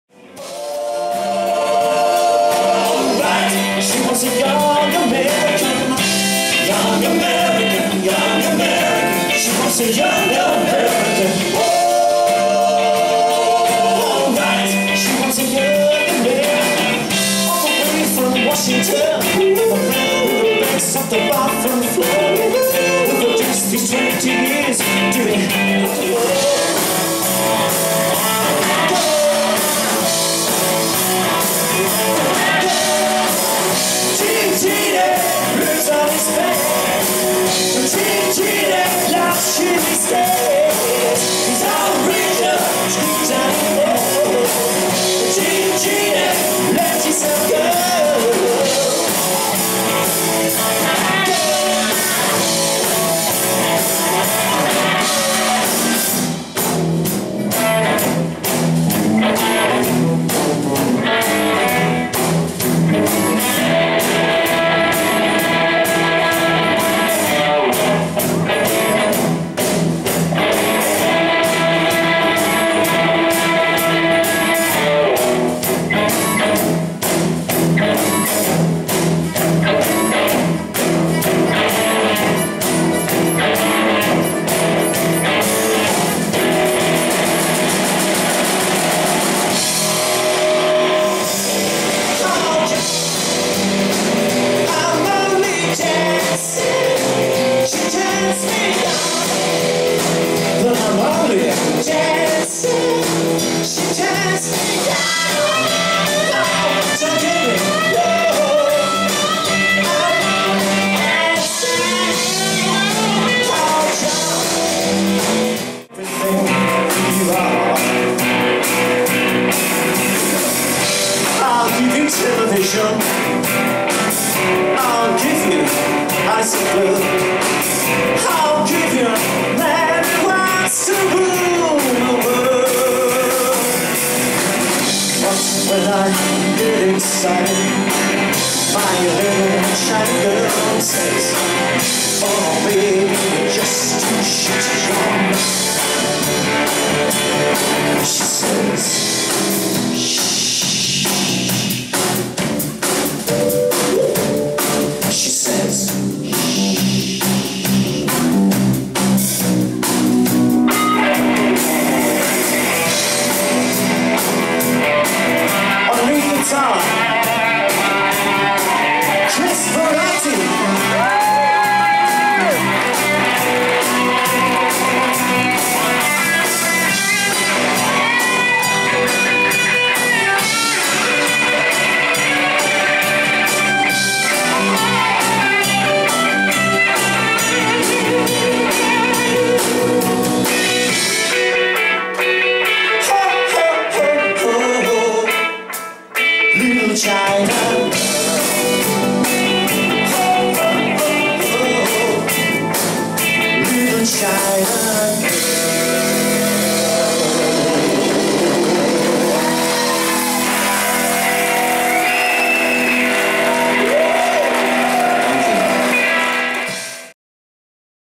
Pop Up Bowie at Thwaites Empire Theatre
Fan video of a quick blast of last nights gig, fantastic night! thanks to everyone who came along!